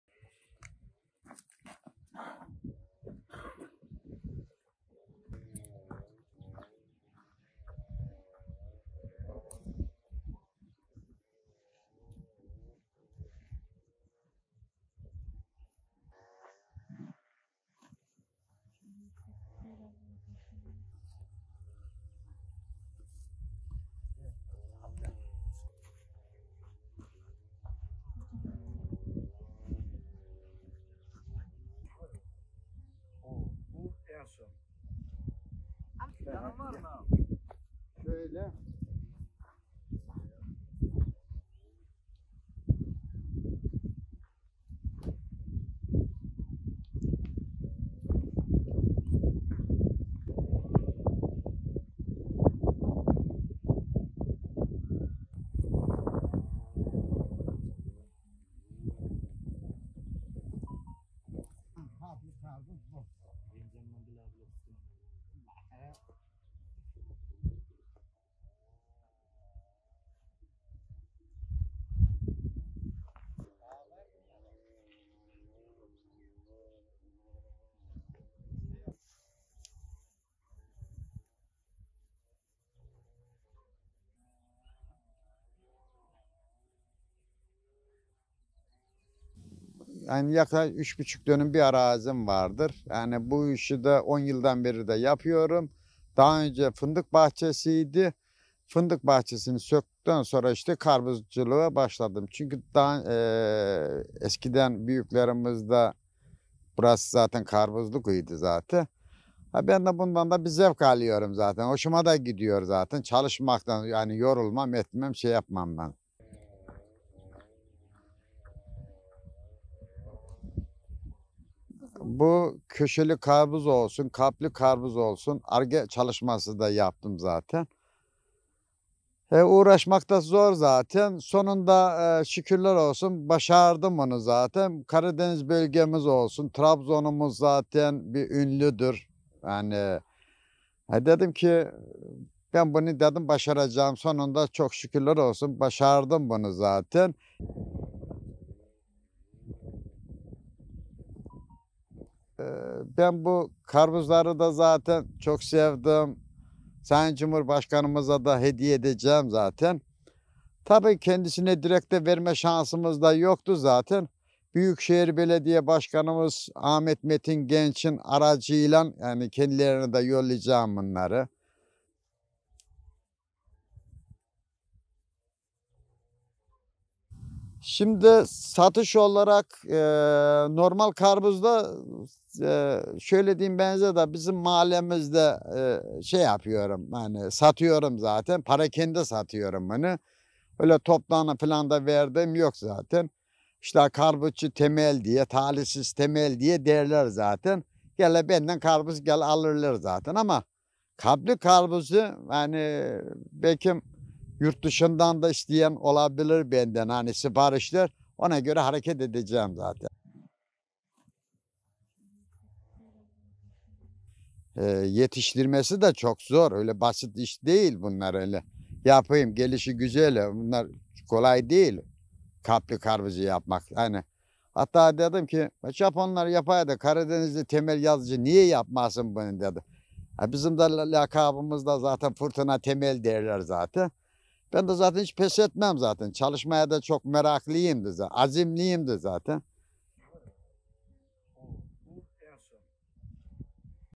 Karadenizli Temel'in talihi şekilli karpuzla değişti
Çevresinde "Talihsiz Temel" olarak tanınan Trabzonlu Temel Yaşar Yazıcı, tarımda yenilikçi yöntemleri kullanarak ürettiği kalp ve kare karpuzlarla iş hayatında başarılı bir sayfa açtı.
Önceden civciv yetiştiriciliği, terlik imalatı, temizlik ürünleri satışının yanı sıra tarımsal üretimde başarısız olan Yazıcı, çevresinde "Talihsiz Temel" olarak tanınıyor.
Ortahisar ilçesine bağlı Akoluk Mahallesi'nde yaşayan 59 yaşındaki Yazıcı, yaklaşık 10 yıl önce kendisine ait 3,5 dönümlük fındık bahçesinden verim alamayınca karpuz üretmeye karar verdi.
Japonya'dan getirttiği tohumlarla "mavi karpuz" yetiştirmek için kolları sıvayan Yazıcı, bu tür karpuzda uğraşlarının karşılığını bir türlü alamadı.
Karşılaştığı talihsizliklere inat yılmadan tarımda yenilikçi girişimlerini sürdüren Yazıcı, bu kez İstanbul'dan getirttiği kalıplarla kalp ve kare şeklinde karpuz üretti. Şekilli karpuz yetiştirmede başarılı olan Yazıcı, bugünlerde sıra dışı hediye arayanların ilgisini çekiyor.
Karpuzların hasadına başlayan 3 çocuk babası Temel Yaşar Yazıcı, karpuz yetiştiriciliği ile yaklaşık 10 yıldır uğraştığını söyledi.
Japonya'da üretilen karpuz türlerinden "mavi karpuz" ile başarısız bir yolculuğu olduğunu dile getiren Yazıcı, yeni işler denemekten asla vazgeçmediğini anlattı.
Yazıcı, karpuz yetiştirmeyi sevdiğini ifade ederek, "Köşeli ve kalpli karpuza yönelik çalışma yaptım, sonunda şükürler olsun başardım." dedi.
Mühendis arkadaşından da öneriler aldığını dile getiren Yazıcı, "Ürettiğim kalpli karpuzlardan çok sevdiğim Sayın Cumhurbaşkanımıza hediye edeceğim. Kendisine direkt verme şansım yok ama Büyükşehir Belediye Başkanımız Ahmet Metin Genç'in aracılığıyla kendilerine yollayacağım." diye konuştu.
Yazıcı, bahçesinden dönüm başına 3 ton karpuz almayı beklediğini, ürünlerine gösterilen talebin kendisini mutlu ettiğini söyledi.
 "Hiç pes etmedim"
Çok büyük hedefleri olmadığını belirten Yazıcı, "Bana 'karpuzcu Temel', 'Talihsiz Temel' derler. Gelir benden karpuz alırlar. Kalpli karpuzları yurt dışından da isteyenler olabilir, ona göre hareket edeceğim." dedi.
Yazıcı, kalp şeklindeki karpuzlara hasat yapmadan önce ilginin başladığını anlatarak, sözlerini şöyle tamamladı:
"Yetiştirmesi de çok zor. Öyle basit iş değil ki gelişi güzel yapılsın. Kalpli karpuz yapmak gerçekten kolay değil ama 'Japonlar yapar da Karadenizli Temel Yazıcı niye yapmasın' dedim. Hiç pes etmedim. Çalışmaya da çok meraklıyım, azimliyim, başardım. Herkes beni sever. Burada halk arasında hatta il dışında her yerde 'Talihsiz Temel' olarak biliniyorum. Vallahi talihim döndü."